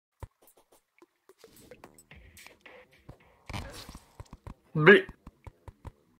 bly-
Tricked my friend to go into a dead mans chest...